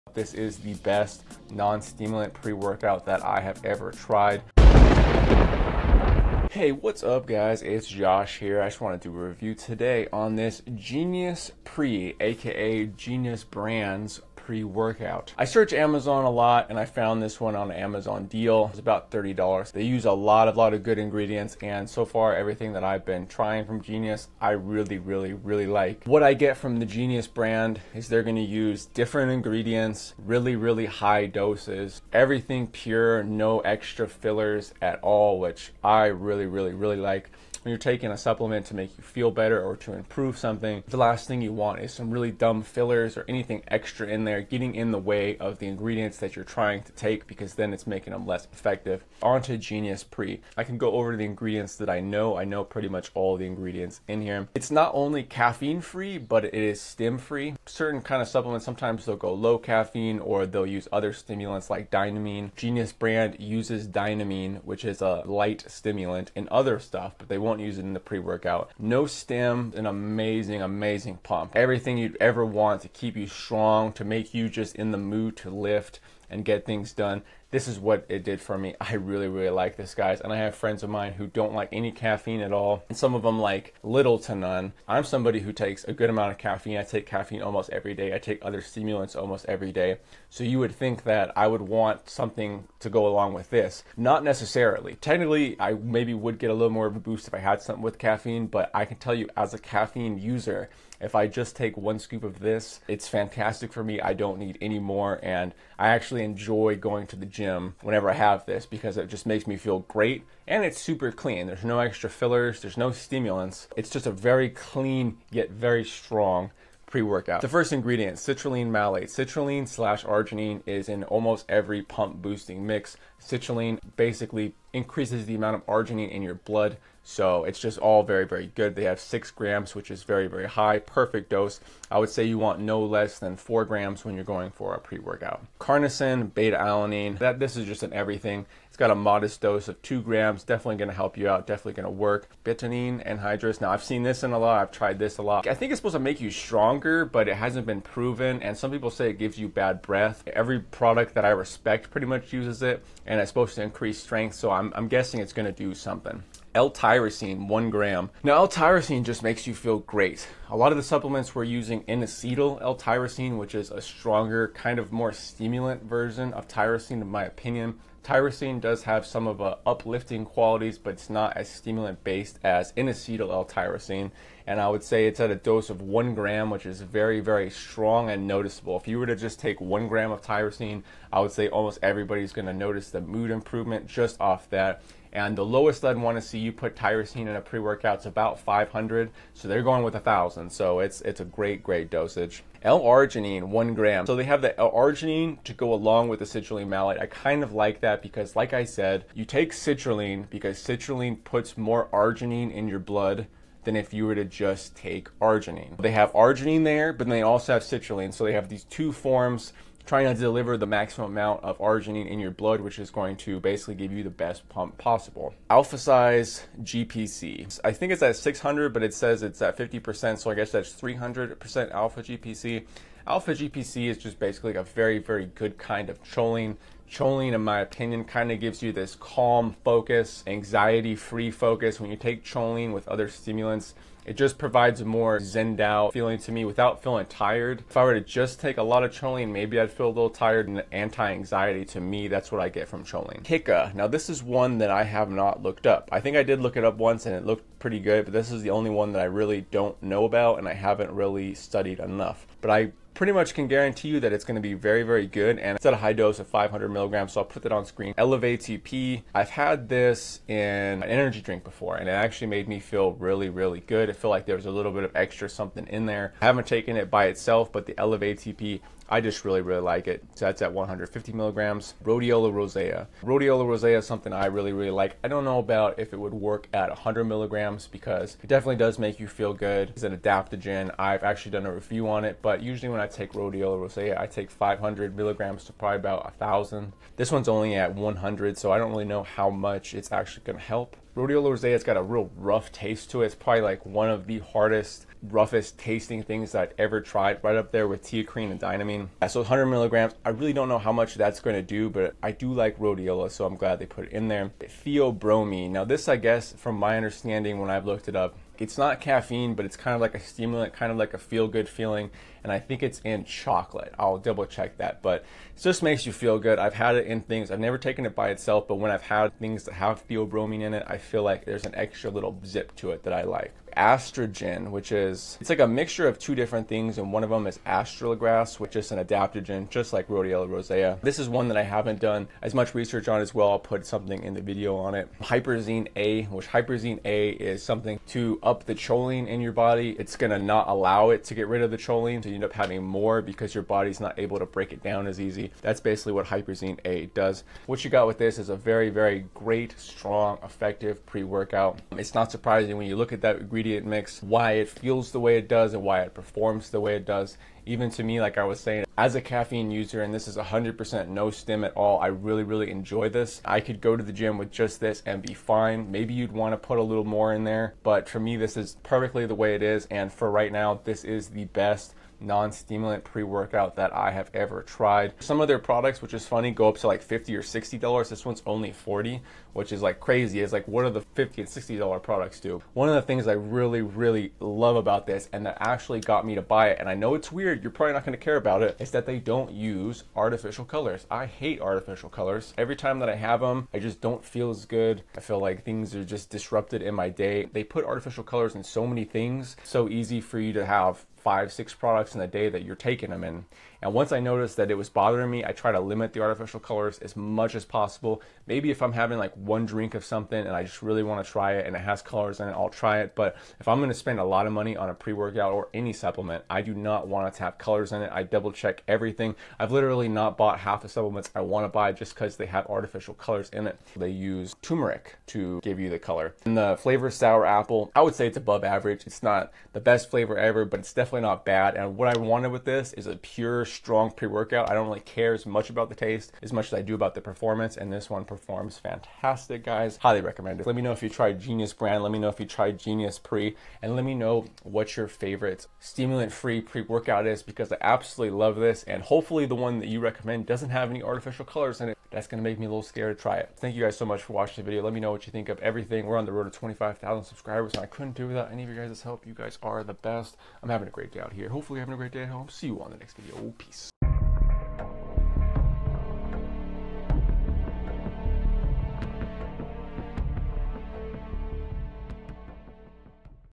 GENIUS PRE Supplement Review (Best Stimulant Free Pre Workout 2022)
Hello Friends! Today I wanted to share my thoughts on one of the best pre-workouts I've tried in a long time, Genius Pre. Have YOU tried Genius Pre or Genius brand supplements?

Genius Pre